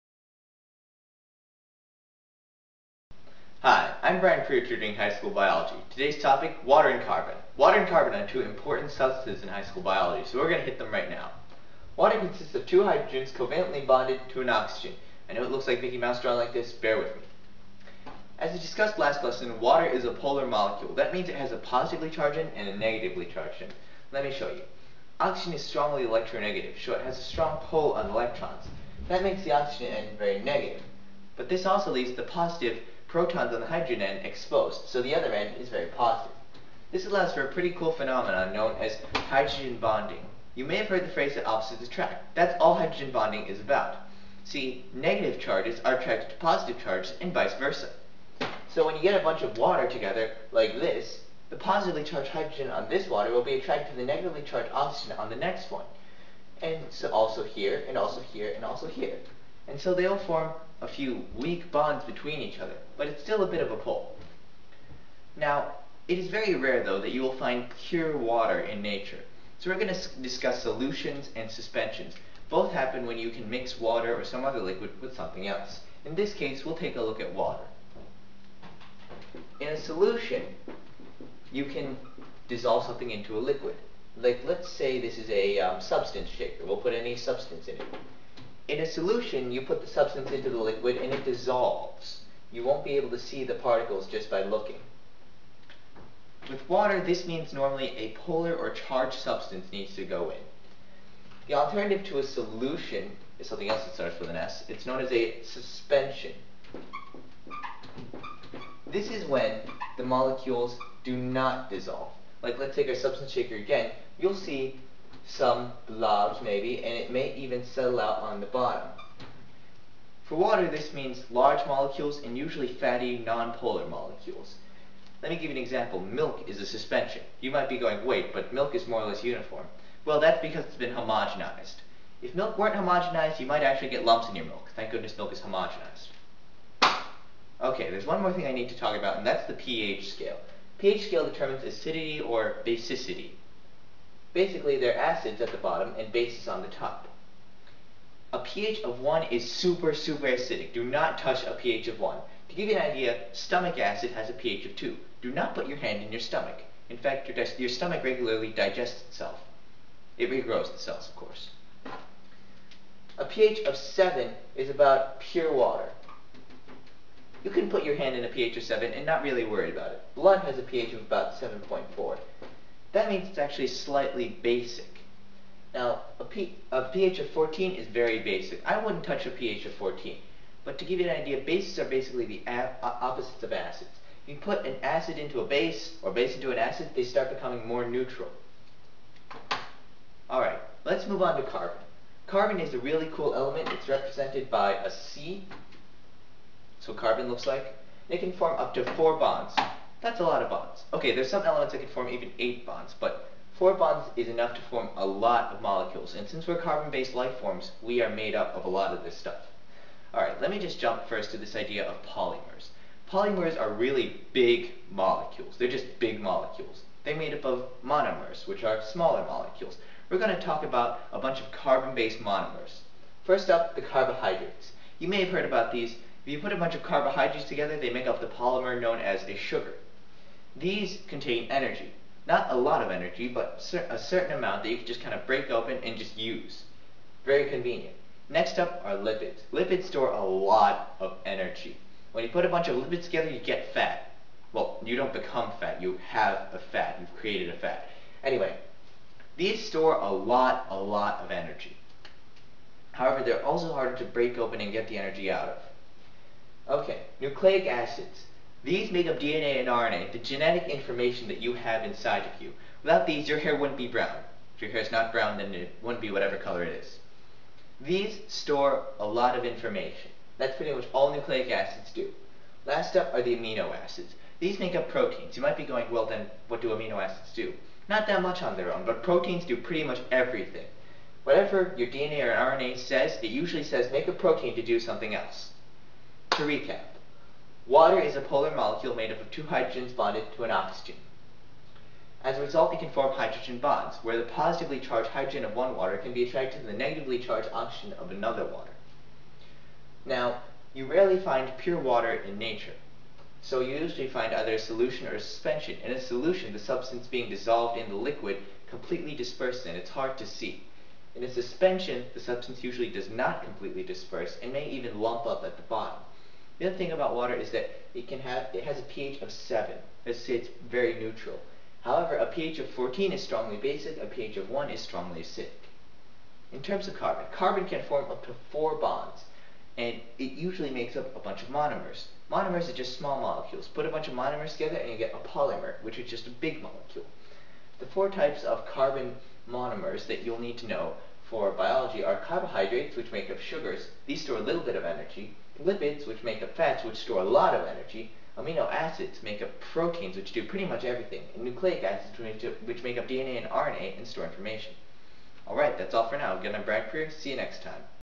03 - Water and Carbon
Oi! You 75%+ water and carbon-based life-form! Surely you want to watch this. Bryant covers the chemical properties of water - hydrogen bonding and polarity - as well as its ability to form solutions and suspensions, and also acidity. For carbon, Bryant covers its common monomers and polymers.